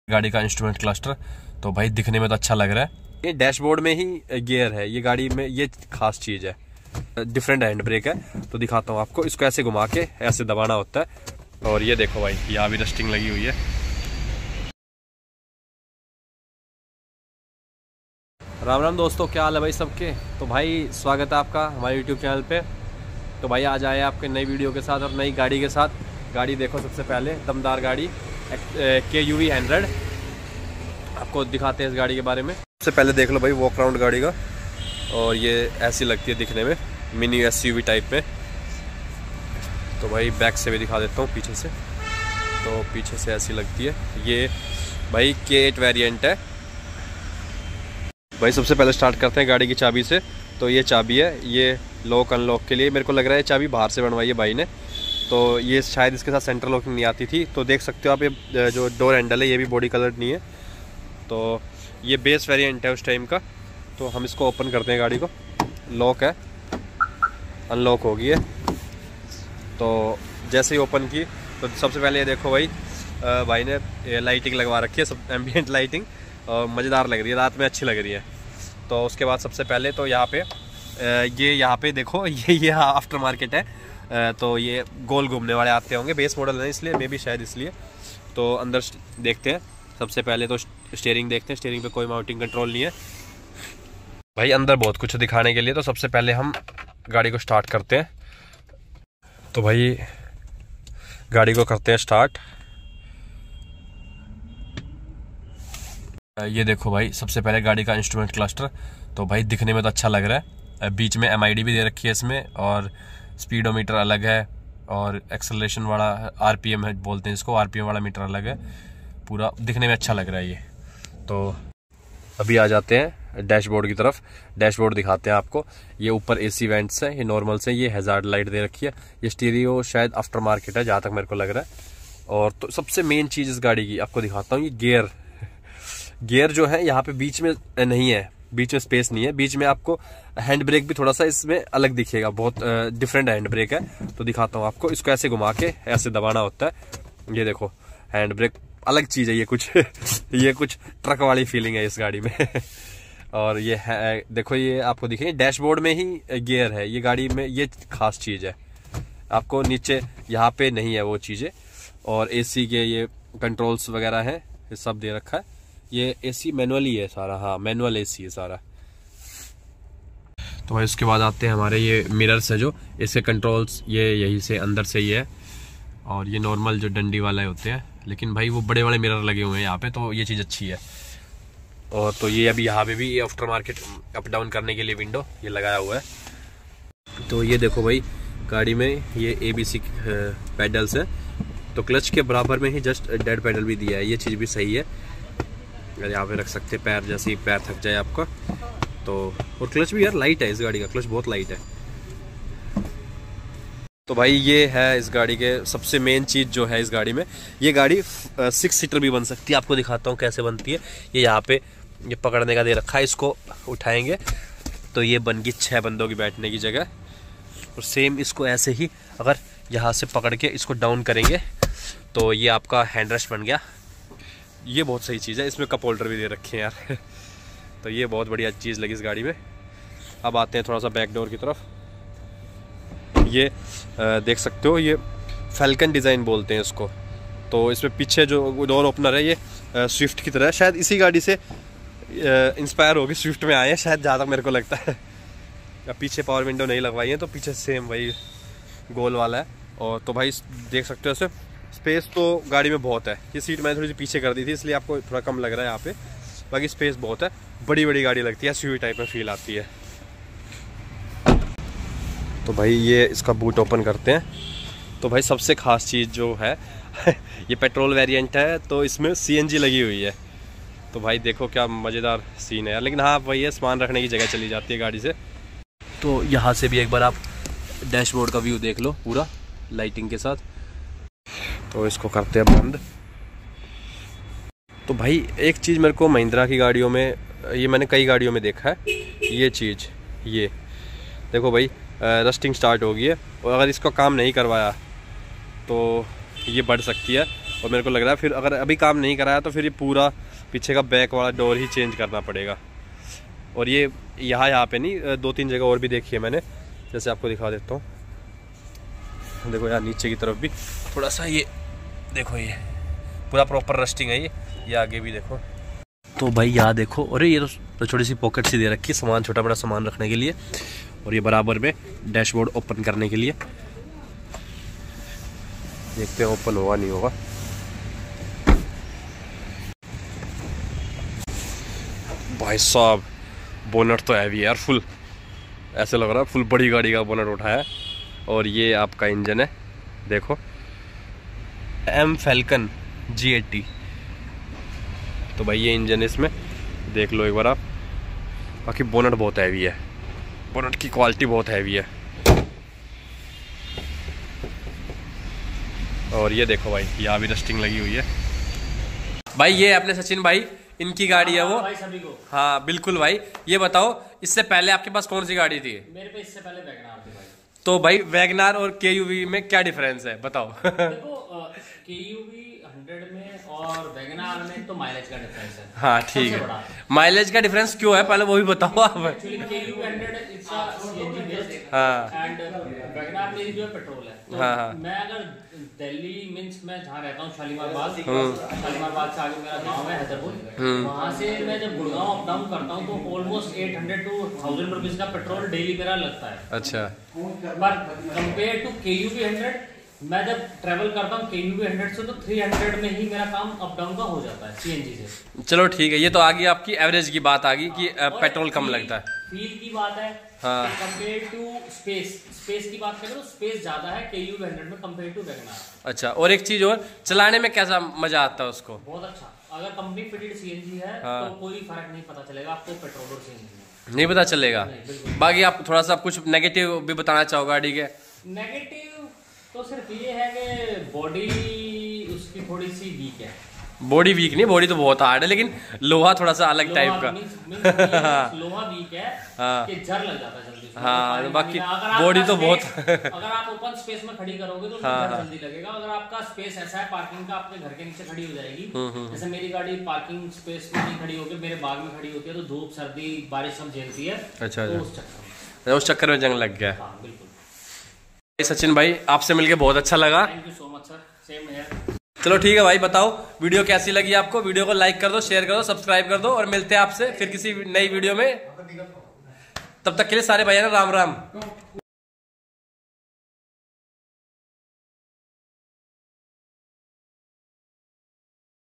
Mahindra KUV100 Review
Mahindra ki Falcon KUV100

Review of the Mahindra KUV100 a mini SUV with Great road presence

Thanks
Kamal Singh